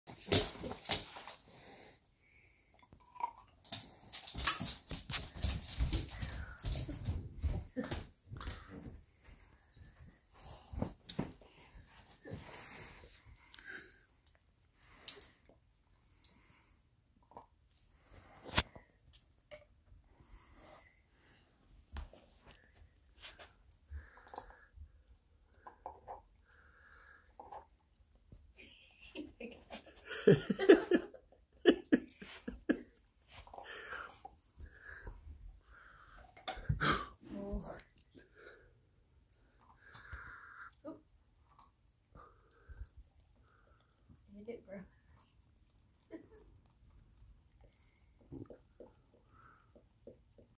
Belle the black and tan coonhound with an egg - take 2
Have you ever given your dog an egg?  Supposedly, they will be gentle with it.